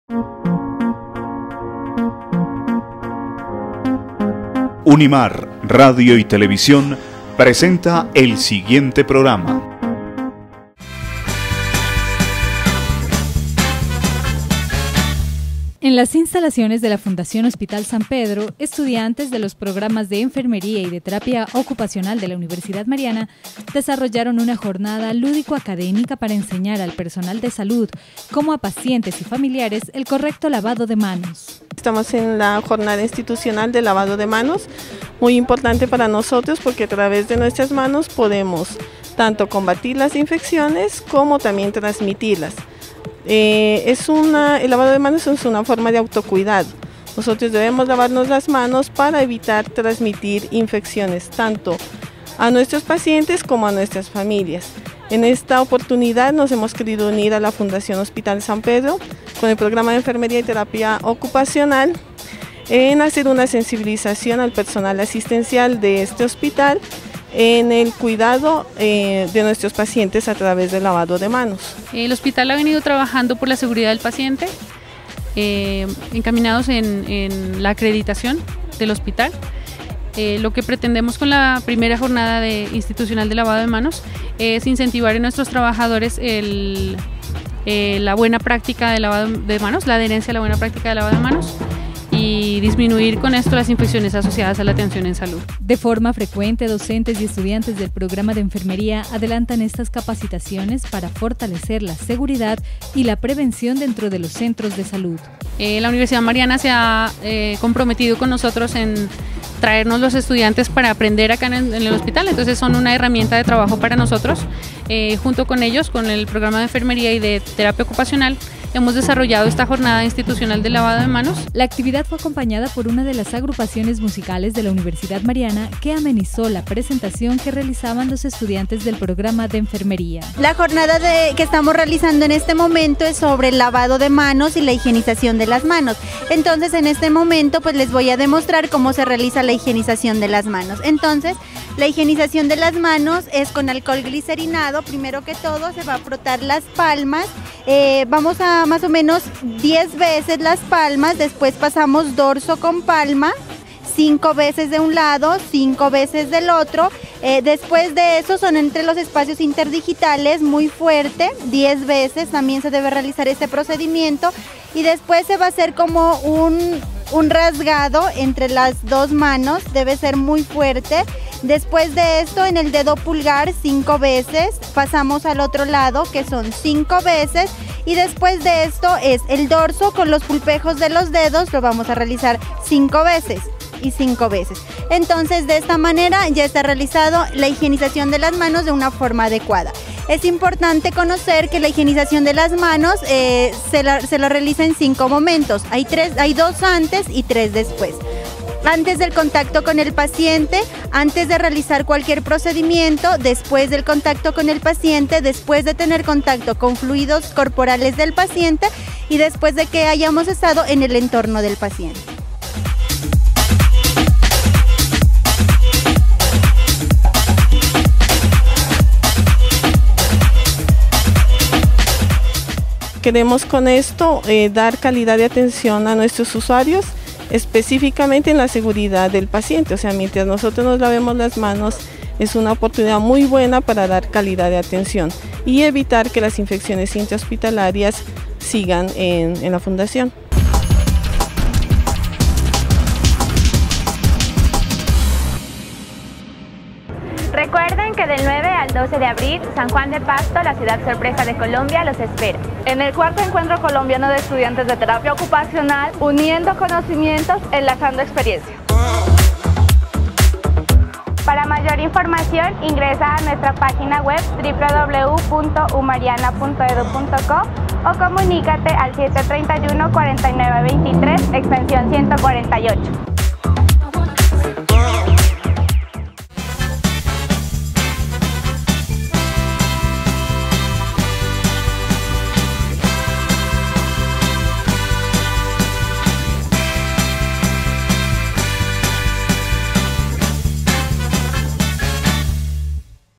Agenda Unimar 02 de Abril 2014
Jornada programa de enfermería en F.H.S.P.
En las instalaciones la Fundación Hospital San Pedro estudiantes del programa de enfermería de la Universidad Mariana desarrollaron una jornada lúdico académica para enseñar al personal de salud, como a pacientes y familiares, el correcto lavado de manos.